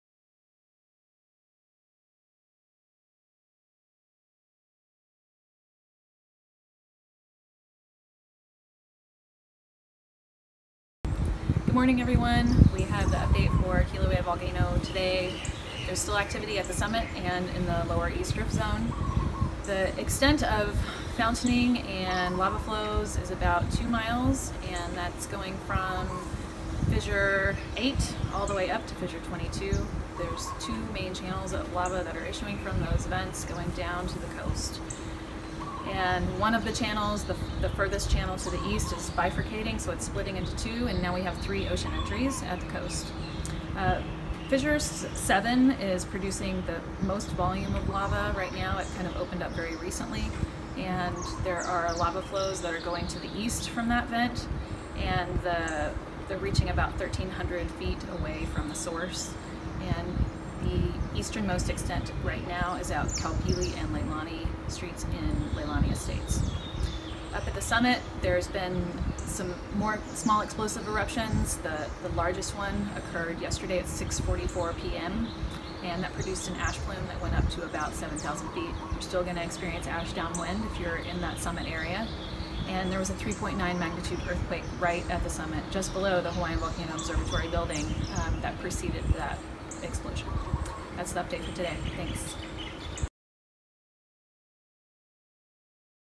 VIDEO: USGS Kilauea volcano update, May 24, 2018
USGS Hawaiian Volcano Observatory status of Kilauea volcano in Hawaii on May 24, 2018